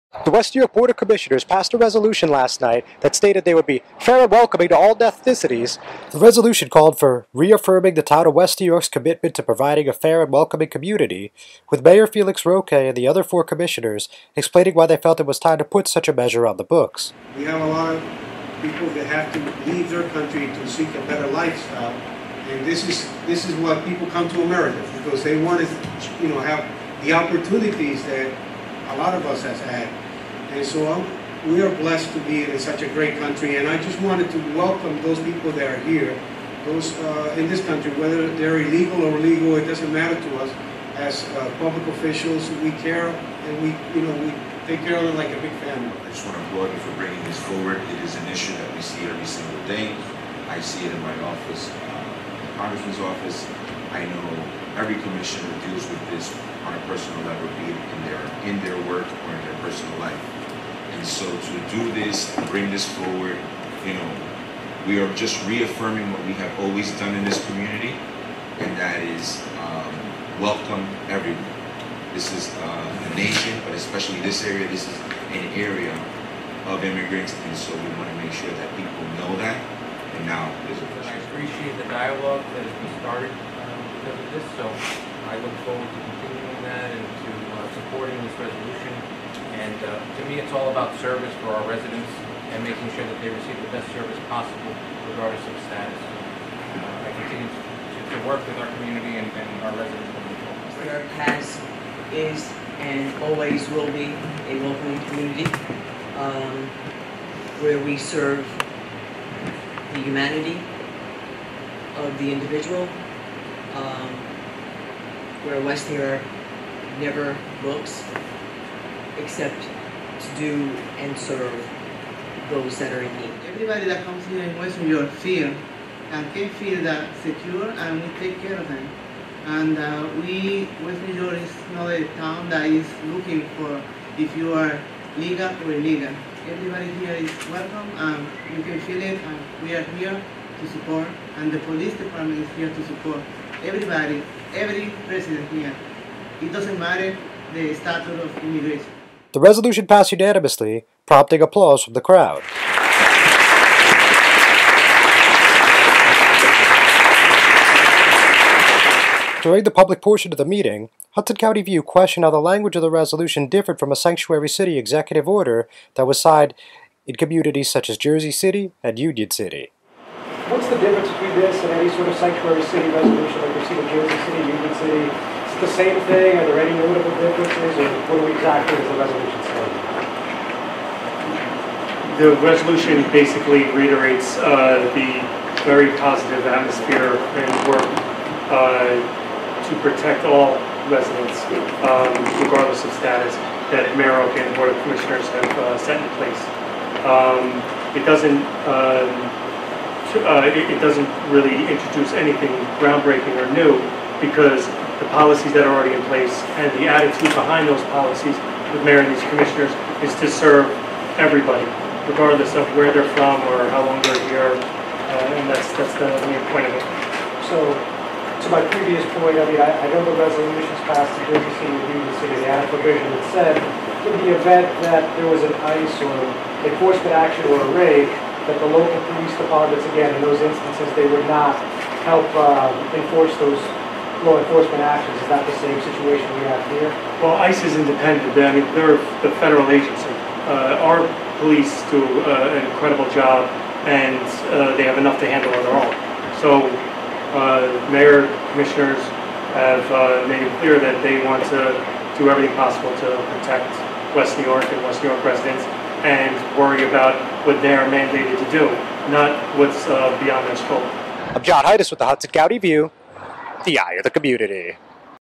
WNY approves measure reaffirming a 'fair and welcoming community'
The West New York Board of Commissioners passed a measure reaffirming a "fair and welcoming community" at last night's meeting.